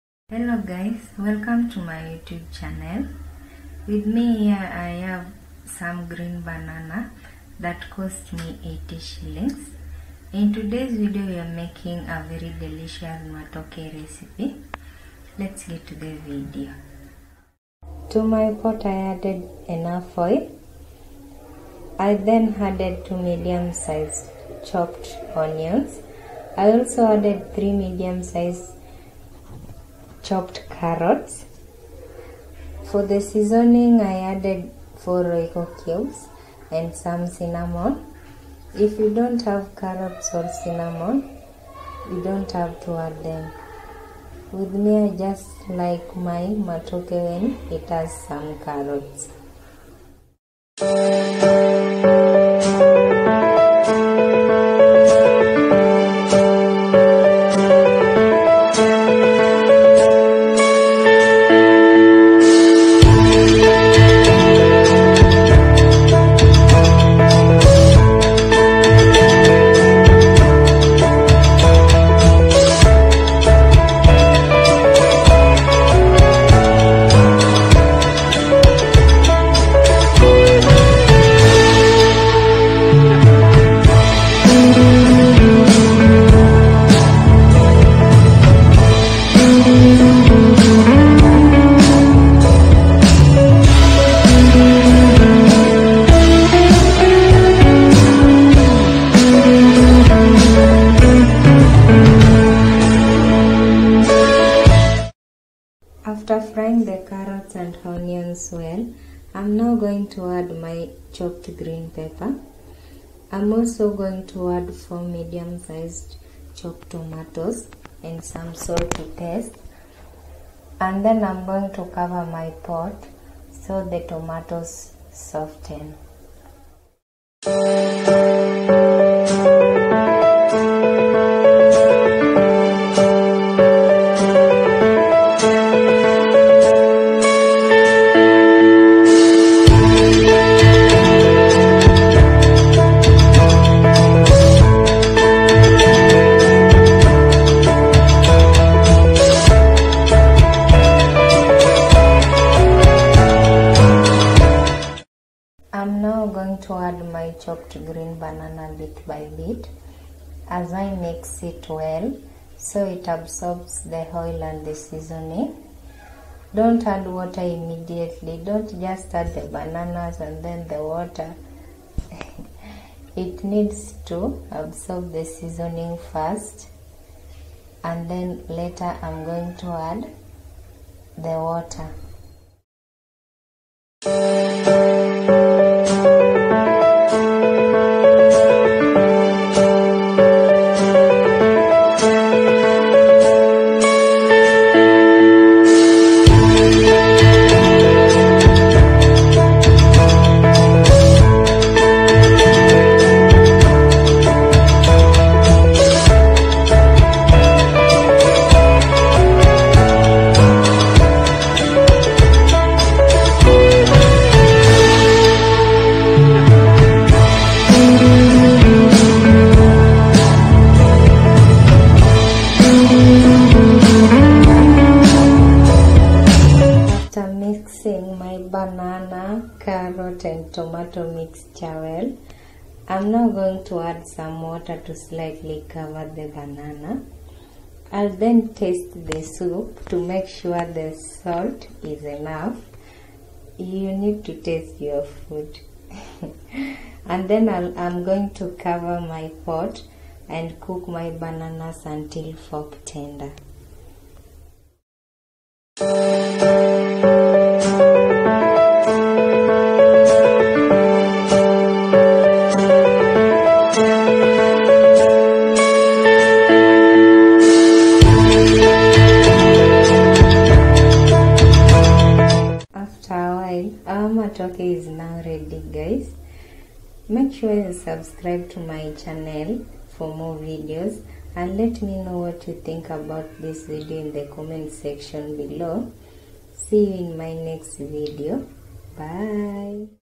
MATOKE RECIPE//GREEN BANANA STEW RECIPE/JINSI YA KUPIKA MATOKE au NDIZI MBICHI/DINNER/ LUNCH RECIPES
In today's video I'm making a very delicious matoke recipe. Matoke is a green banana recipe that is staple in most part of the country. I love making matoke in my house since my son loves it a lot. It's delicious and good for toddlers.